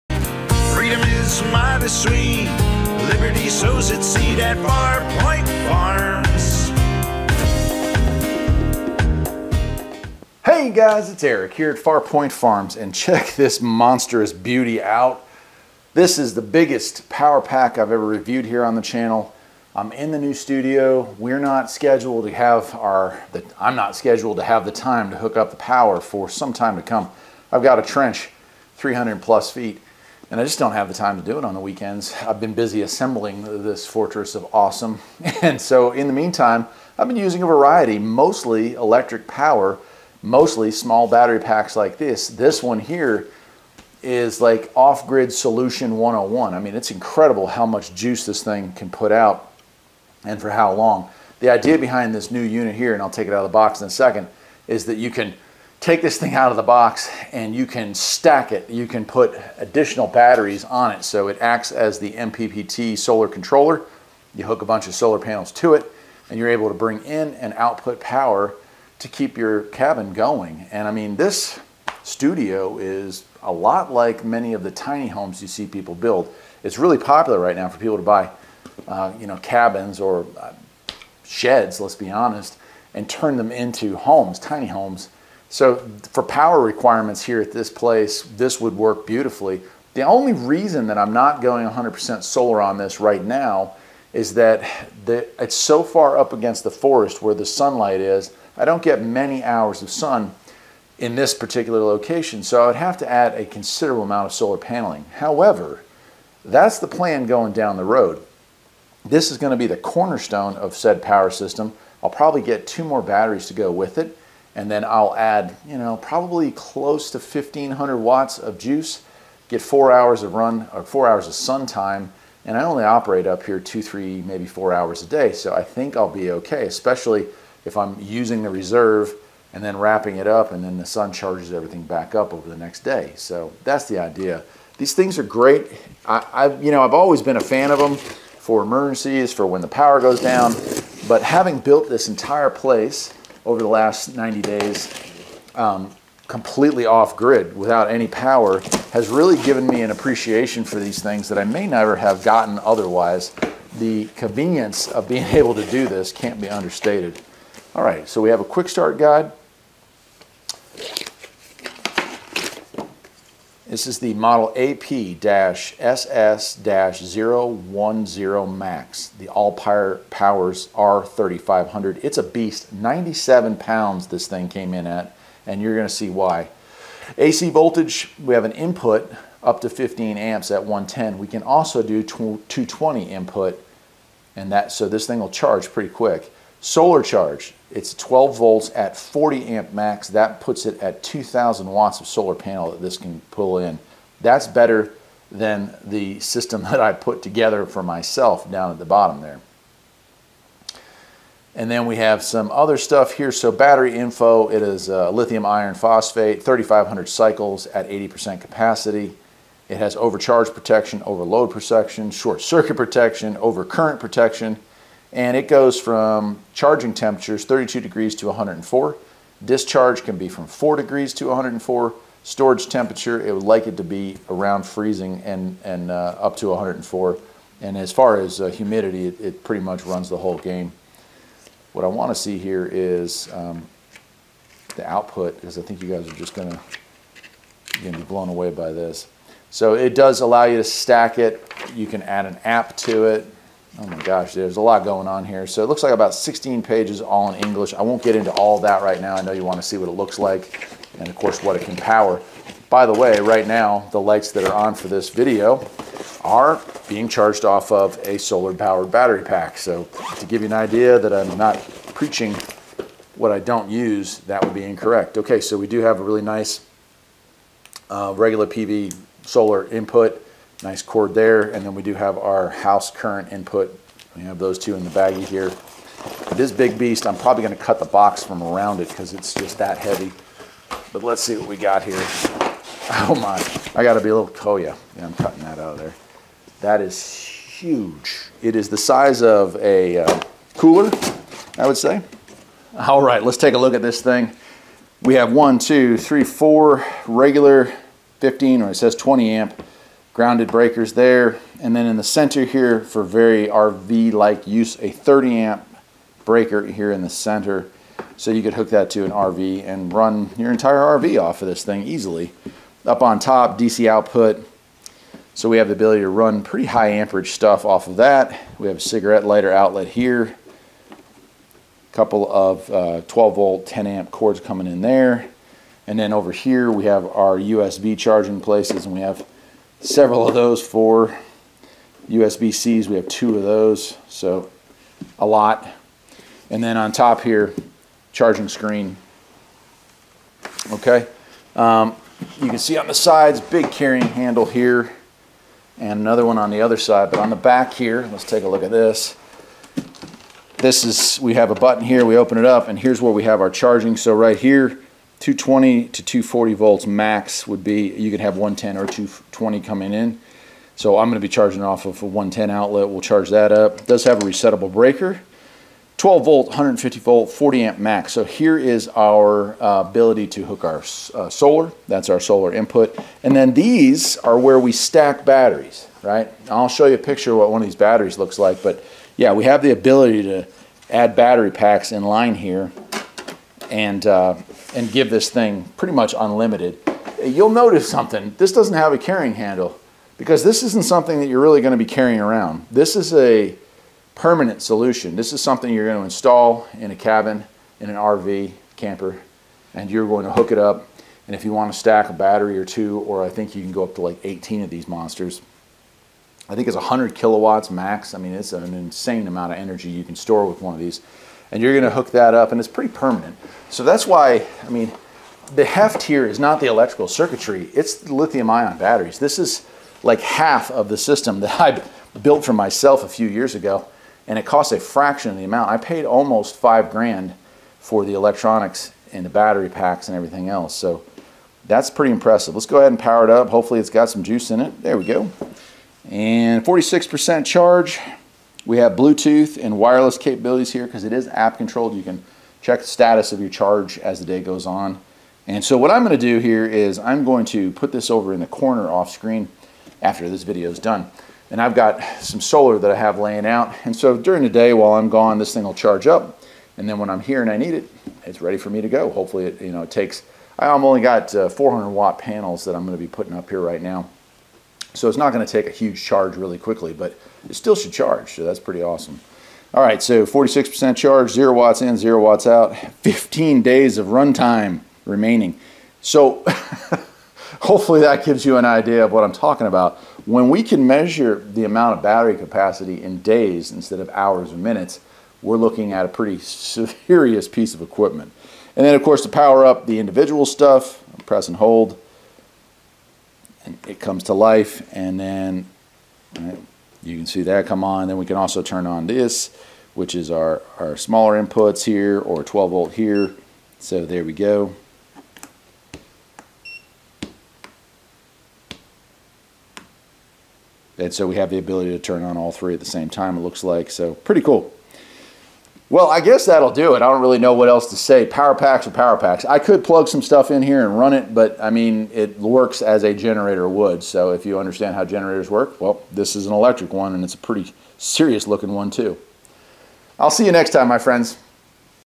Allpowers R3500 Mega Powerpack. This literally is the same as my entire solar setup, but compact.
Wow. In 2020 I started the process of moving my home off grid. As of today, we are sitting at just under 50% of our homestead being powered by Solar. I spent a lot of time finding the right charging system, batteries, and inverter.
 Now?
 Now  you can buy a unit like this and only have to add solar to make the magic happen. I I've been using this and 400 watts of solar up at the new studio to power things while I work on it and I am super impressed.
and save 15% with this code:  R6FF15

It's not cheap, but it is less expensive than the same system I setup myself. It's a all in one solution!